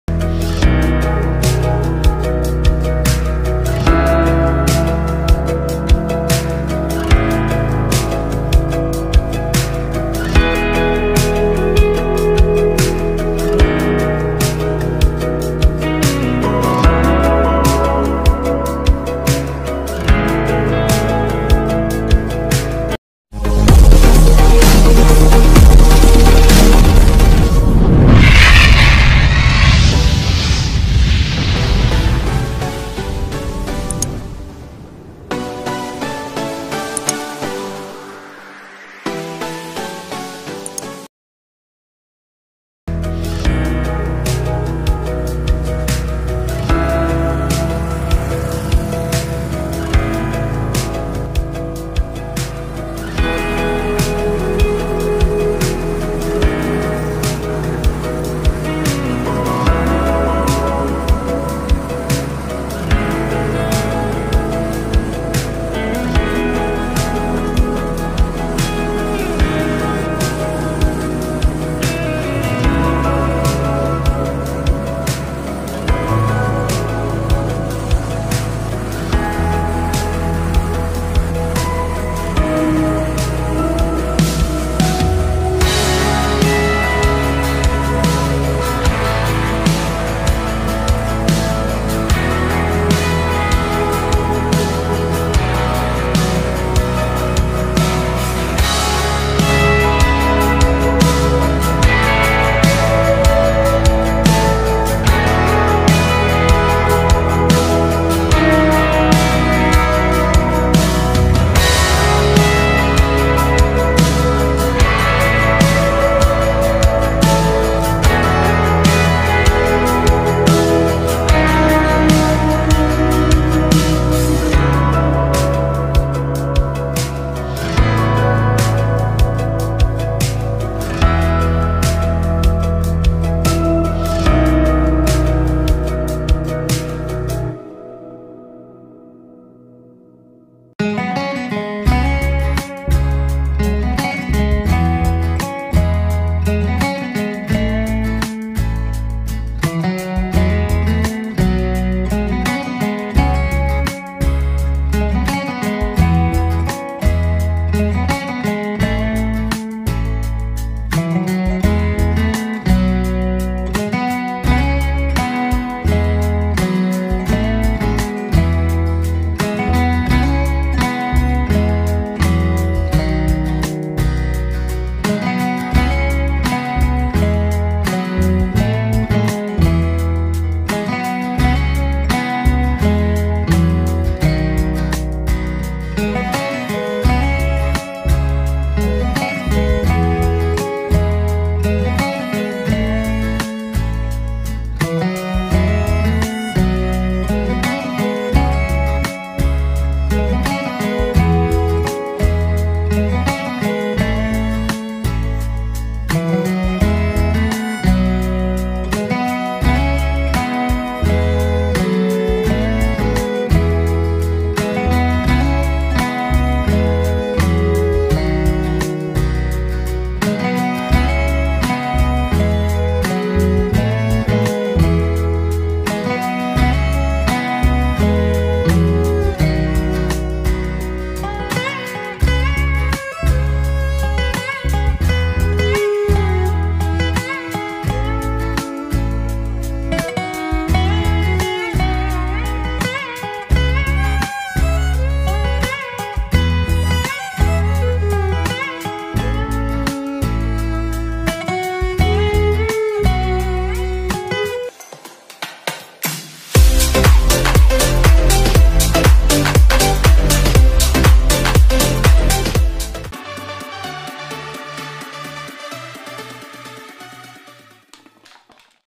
LONTAR SEWU WIS TAK LEWATI
Edu Wisata Lontar Sewu.
Desa Hendrosari, Menganti, Gresik.

Pohon lontar adalah sejenis palm yang buahnya seperti kelapa tapi kecil kecil dan biasanya bunga lontar ini disadap dan airnya dijadikan minuman yang sering dikenal dengan Legen atau air nira.

Semoga terhibur.
Terima kasih.